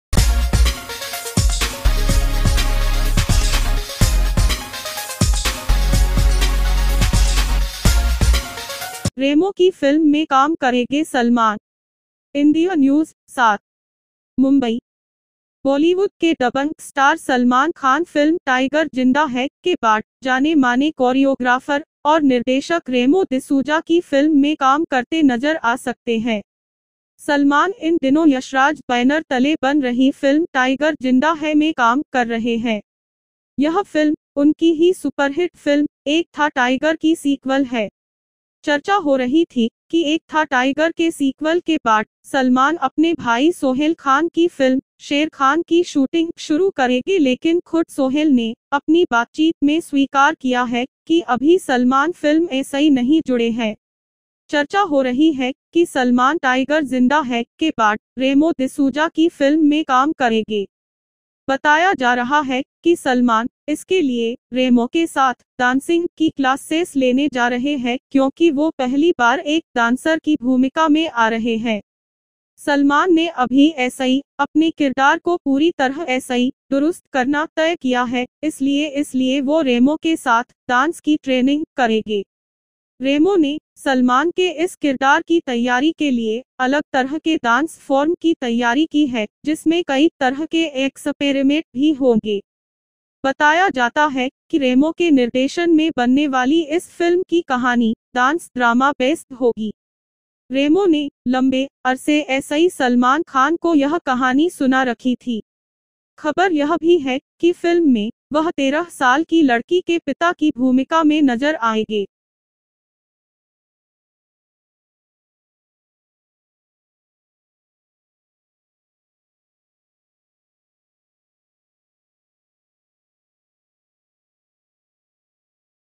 रेमो के साथ काम करेंगे सलमान खान | डांस की ट्रेनिंग कर रहे है । जानिये और कुछ बाते!
Upcoming Movie Salman Khan with remo Sir